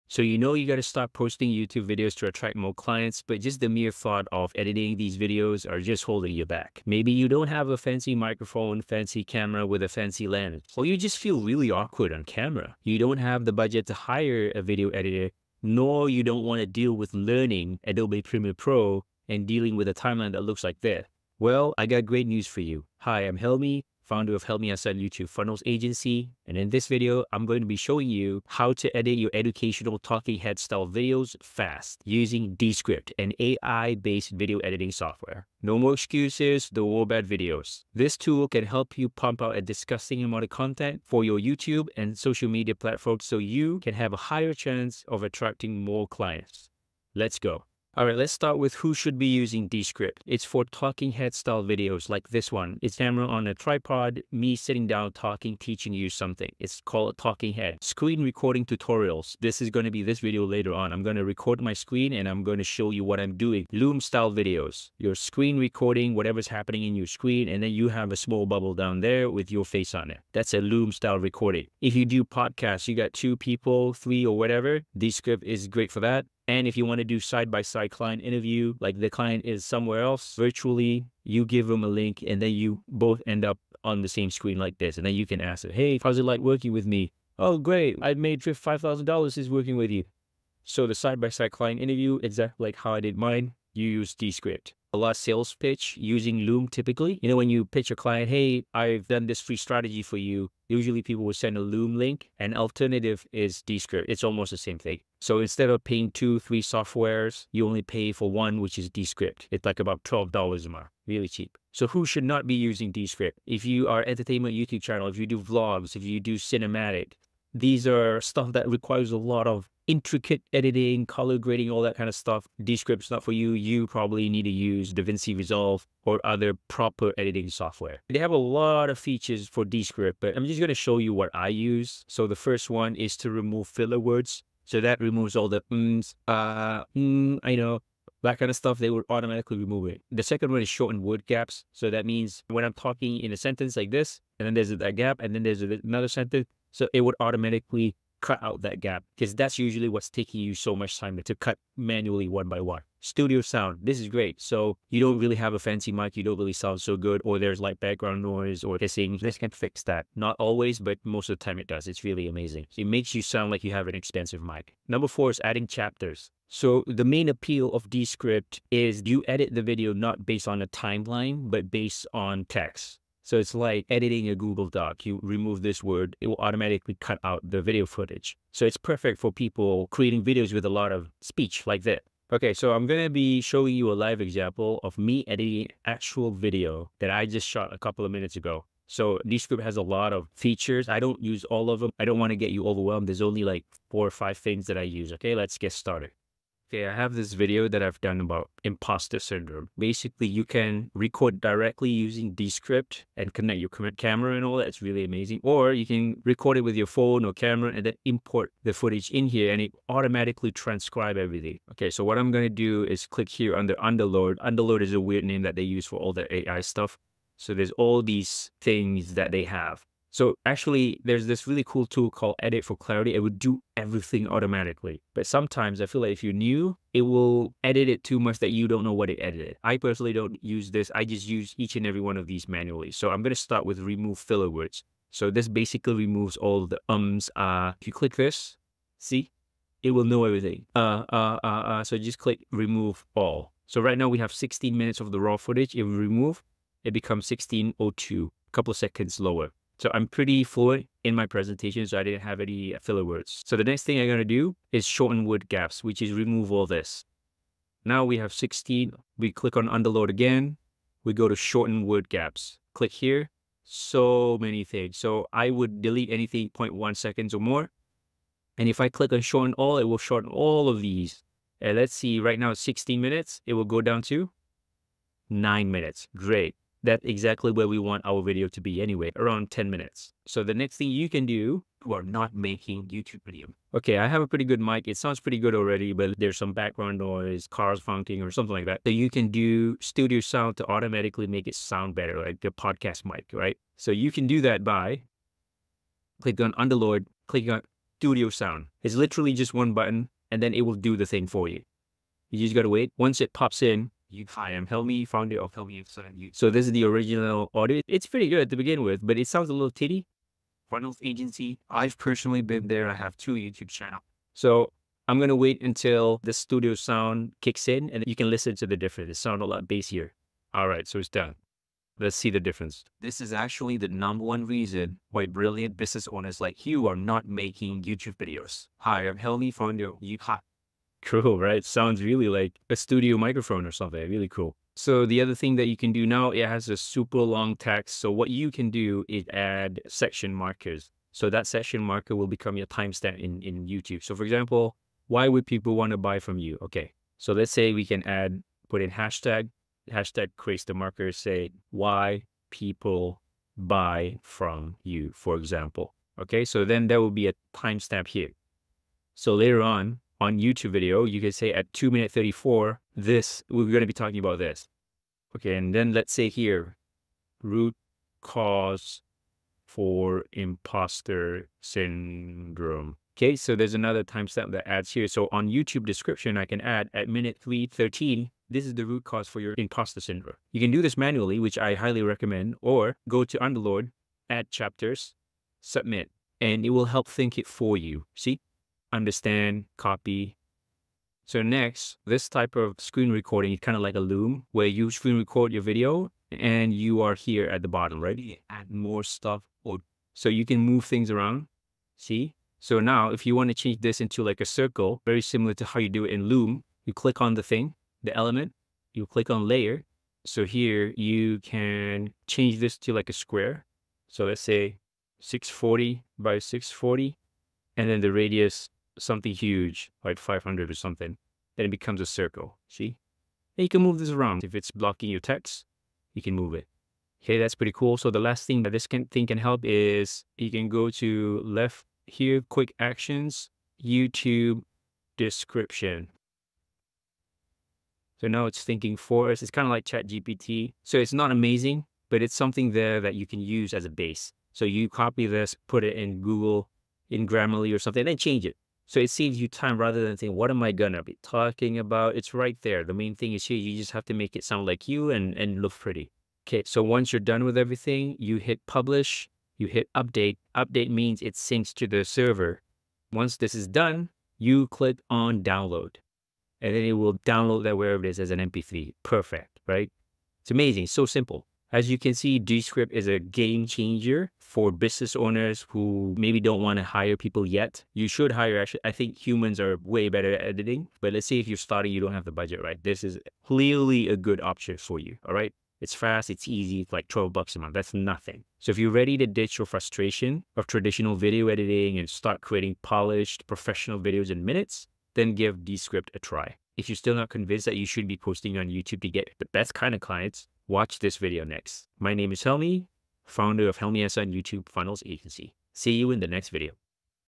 The New Way to Edit YouTube Videos for Your Business
Feeling held back by video editing? I’ll show how I streamline the editing process using Descript, an AI-based video editing software.

Learn how to remove filler words, shorten word gaps, enhance audio quality, and add chapters with ease, making it perfect for talking head-style videos, screen recordings, and podcasts.

Say goodbye to complex timelines and expensive software, and hello to producing professional videos quickly and affordably.

0:00 - Video starts
00:47 - Who Should Use Descript?
01:51 - Who Should Not Be Using Descript
02:08 - Key Features of Descript
03:02 - Live Descript Editing Demonstration
03:55 - Removing Filler Words
04:13 - Shorten Word Gaps
04:40 - Studio Sound
05:34 - Adding YouTube Timestamps
06:31 - Create Loom-Style Screen Recording
07:07 - YouTube Description with AI
07:36 - How to Publish Your Video
07:51 - Final Thoughts and Recommendations